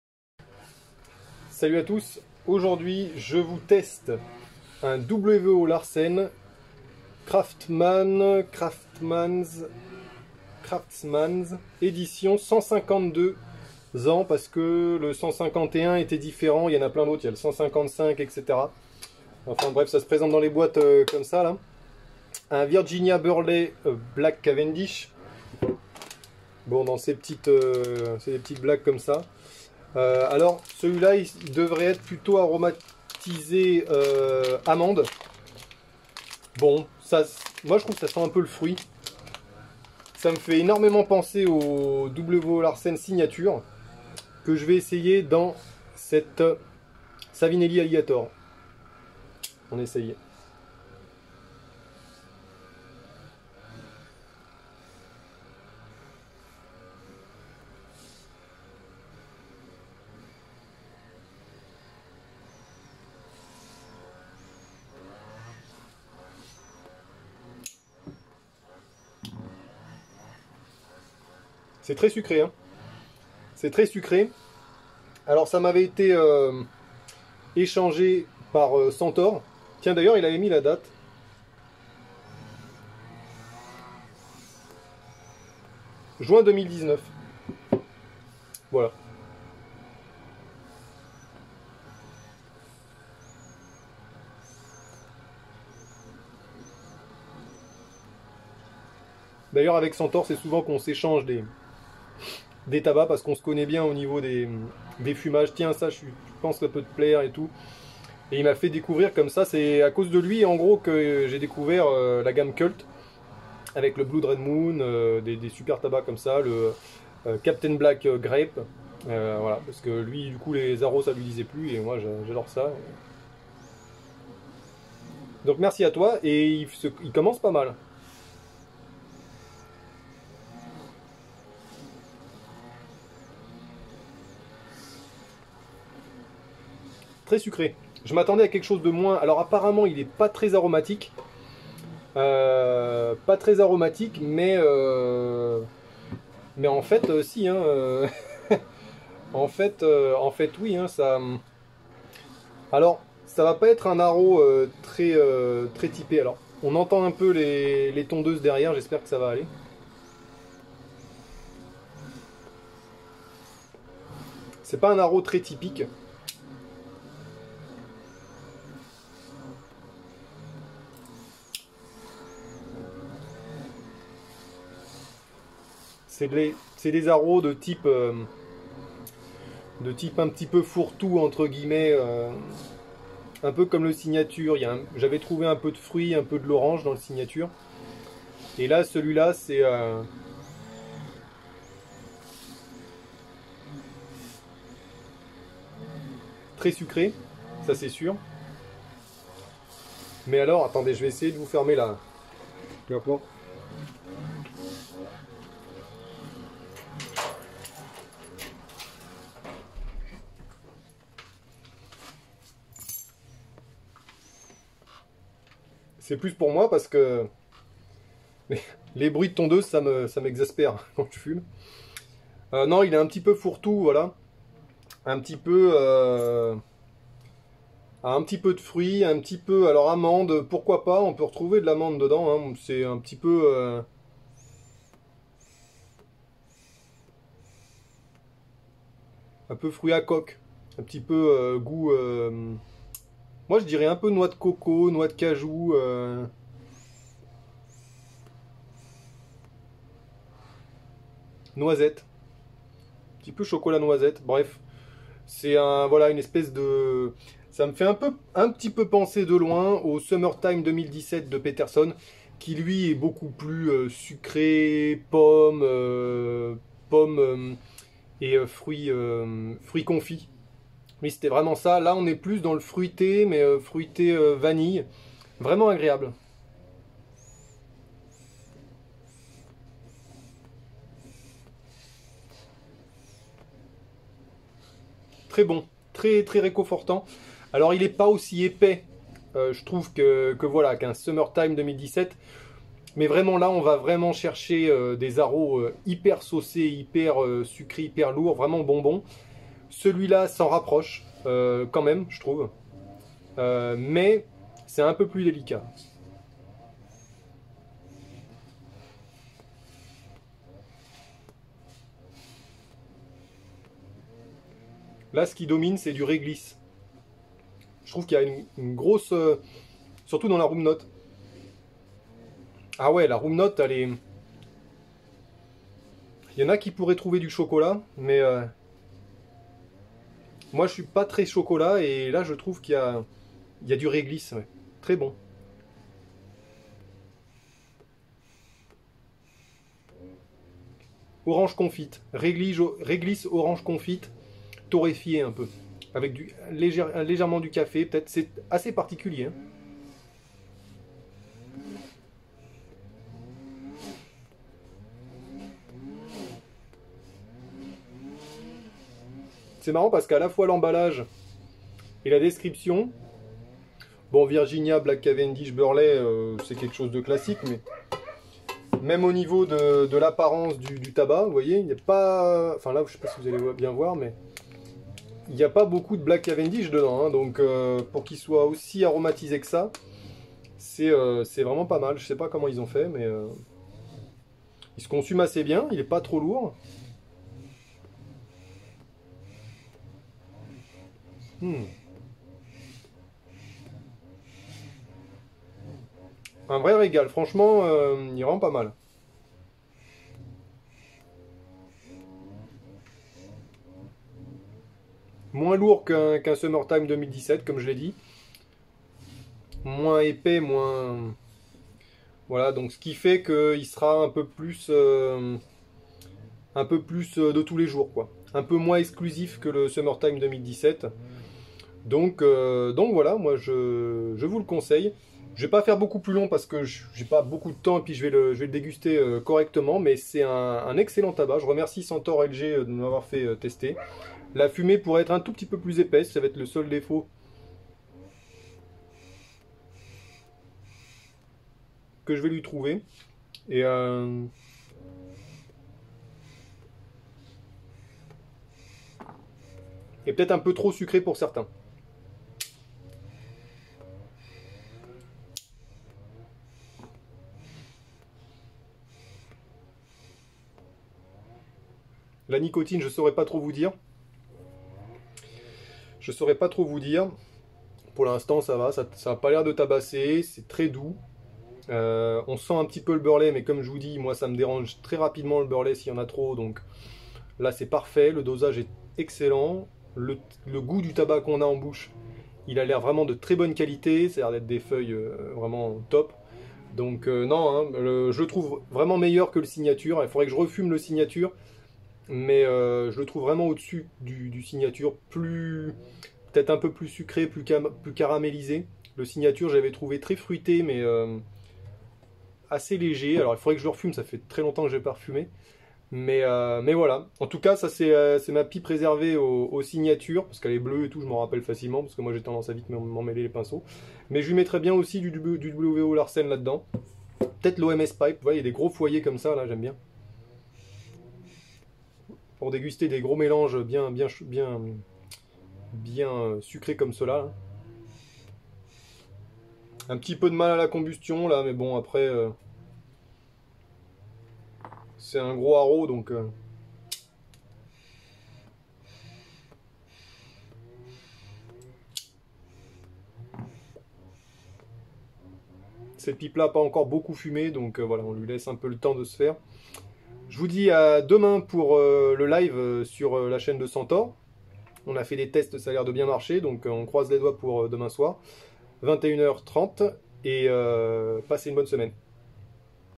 Wo Larsen Craftsmans 152
Très sympa et bien saucé ! Ambiance fruits et réglisse pour moi !